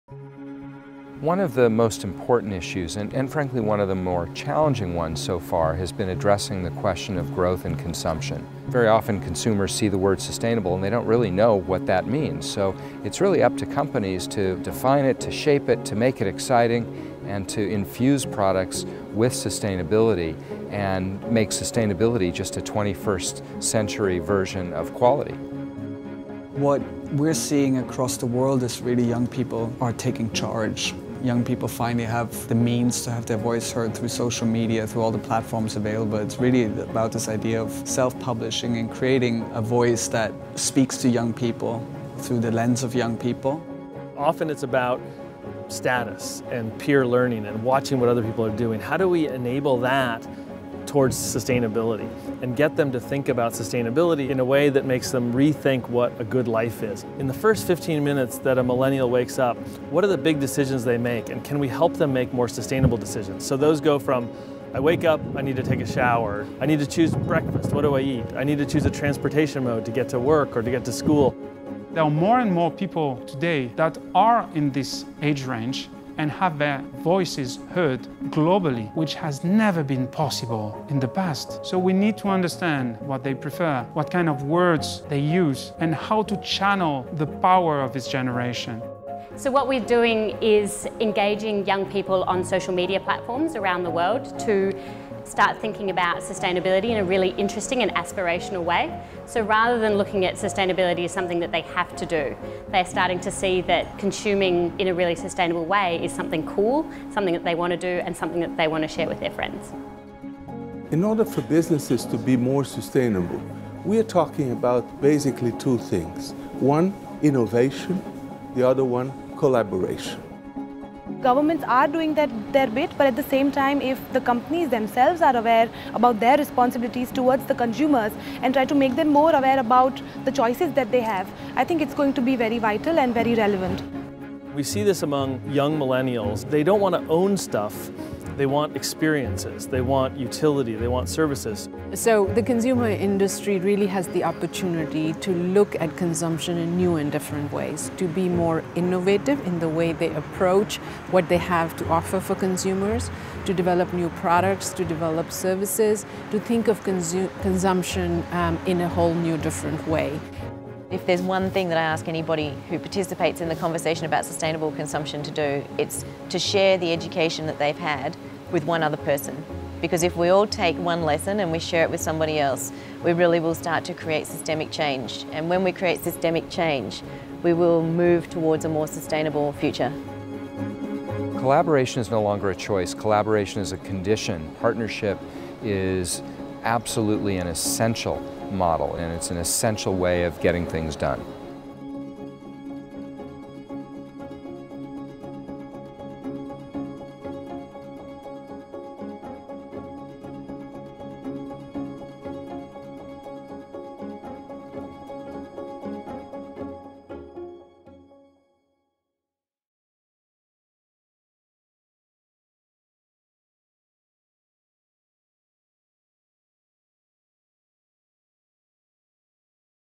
Engaging Tomorrow's Consumer
Let's make sustainability a 21st century version of quality, says Aron Cramer, CEO of Business for Social Responsibility, USA. He opens this call for companies to encourage young consumers to make more sustainable choices.